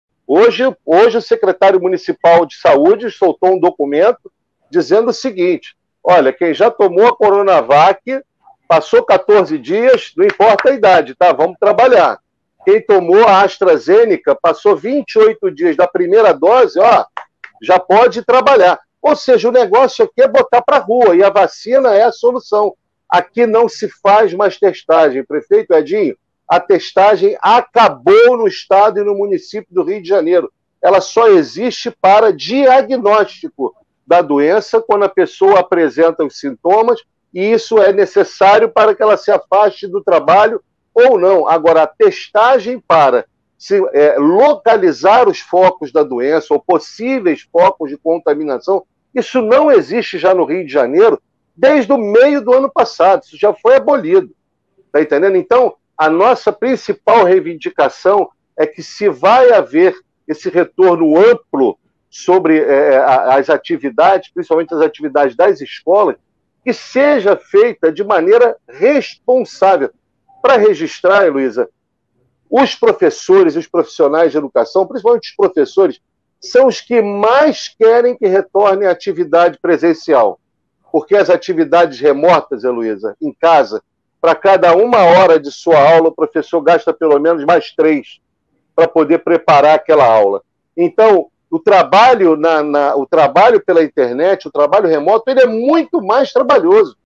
O caos no Rio de Janeiro | Minuto Com Helô
É necessário esperar o efeito completo da imunização contra a Covid-19. Se os professores vão voltar às suas salas, é preciso voltar com responsabilidade.